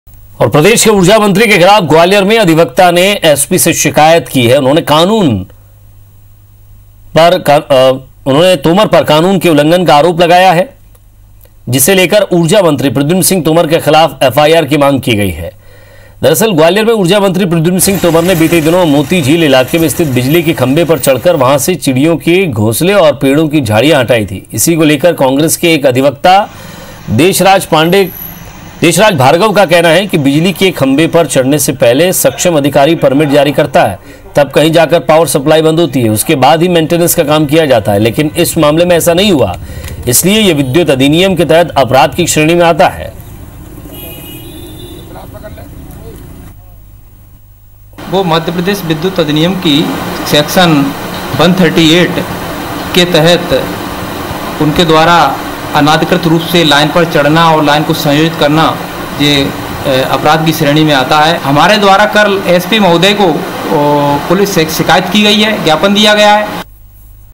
Gwalior में ऊर्जा मंत्री Pradhuman Singh Tomar की SP से शिकायत। बिजली के पोल पर चढ़कर की थी सफाई
IBC24, Gwalior में ऊर्जा मंत्री Pradhuman Singh Tomar की SP से शिकायत, बिजली के पोल पर चढ़कर की थी सफाई